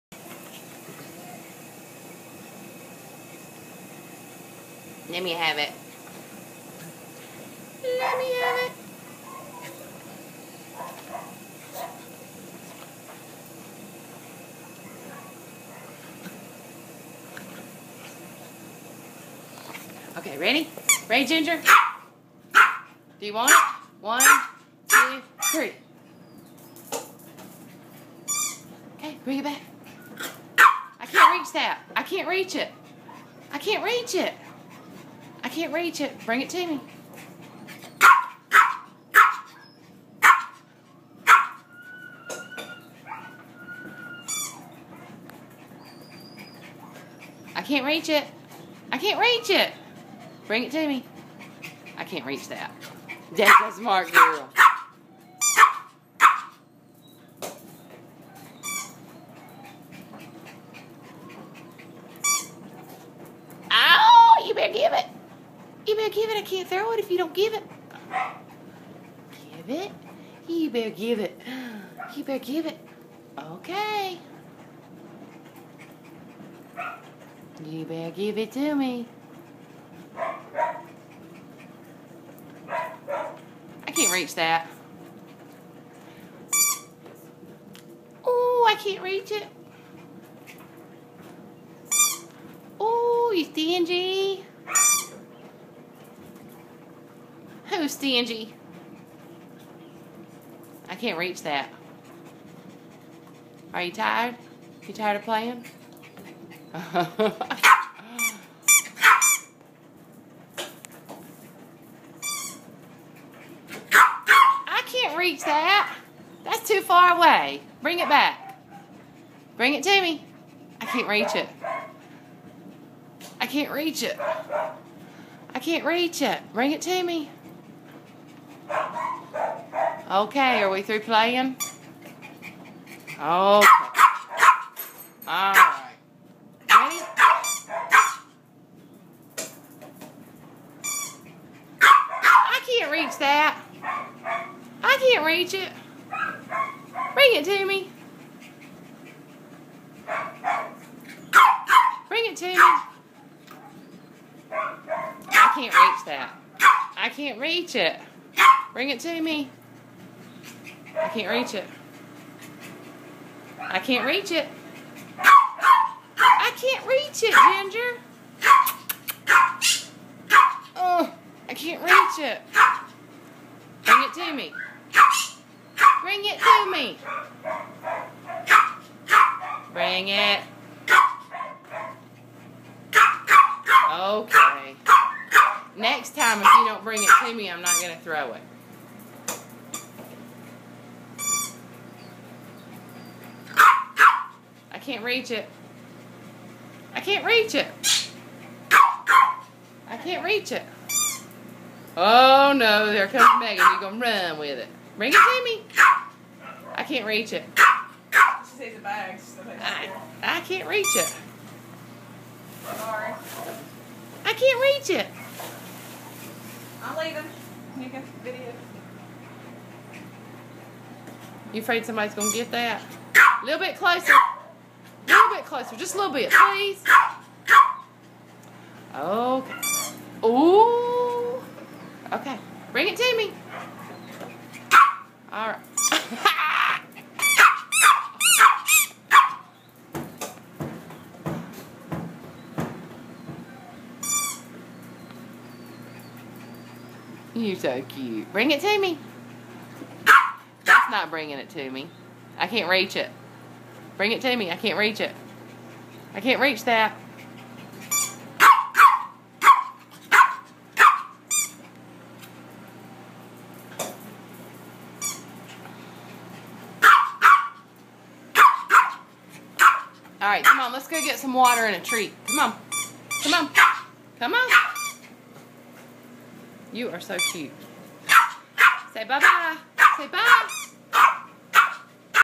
Ginger playing fetch
Boarding at Southwoods Animal Hospital. Greensboro NC